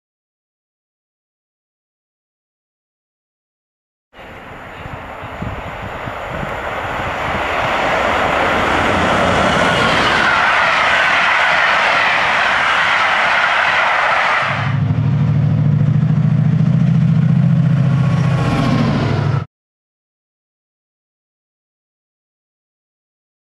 UK Railtours The Conway Explorer
90034 on UK Railtours The Conway Explorer, 1Z77 0639 London Euston to Llandudno, seen passing Nuneaton. Bonus 68010 on 0650 Crewe Gresty Bridge (Drs) to Wembley Lmd, nearly an hour early.